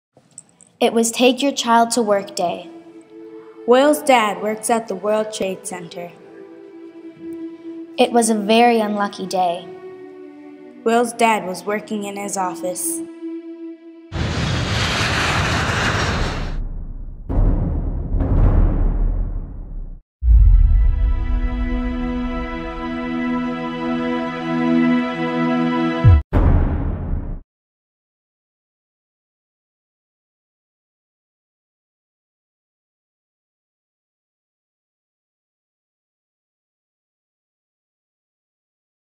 We All Fall Down 22
A book trailer for Eric Walters' We All Fall Down from Room 22.